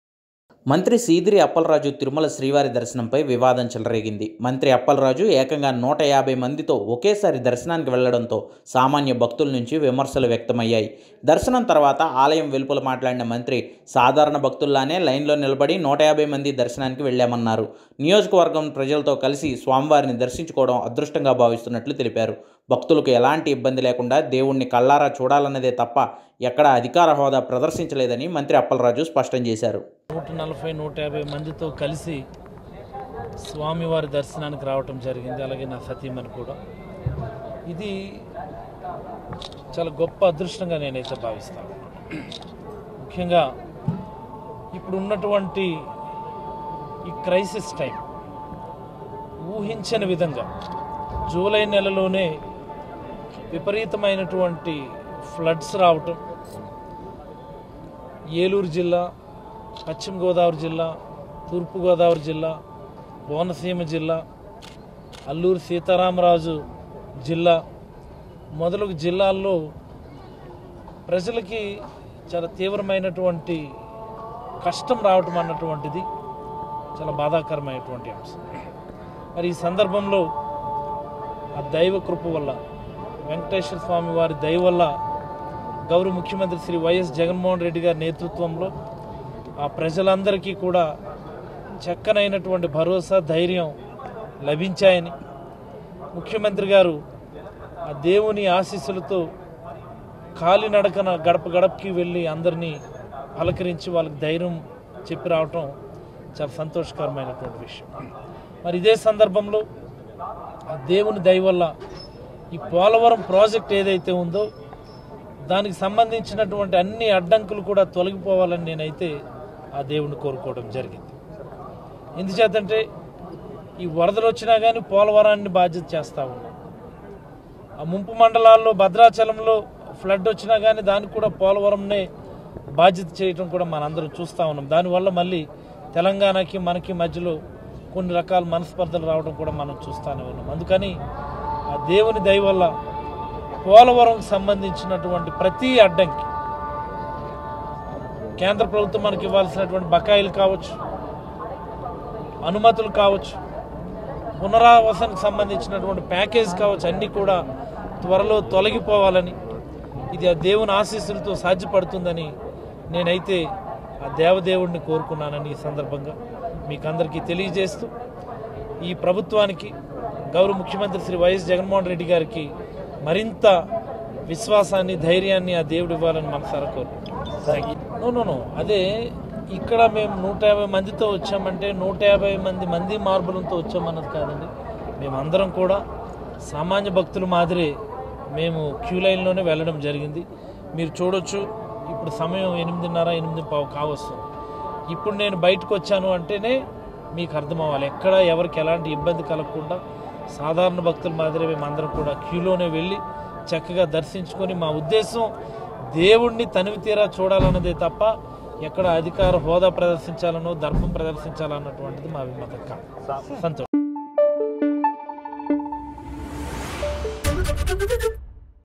Minister Appalaraju Tirumala Darshan : తిరుమల శ్రీవారి దర్శనాలపై వివాదం| ABP Desam
మంత్రి సీదిరి అప్పలరాజు తిరుమల శ్రీవారి దర్శనంపై వివాదం చెలరేగింది. మంత్రి అప్పలరాజు ఏకంగా 150మంది ఒకేసారి దర్శనానికి వెళ్లటంతో సామాన్య భక్తుల నుంచి విమర్శలు వ్యక్తమయ్యాయి.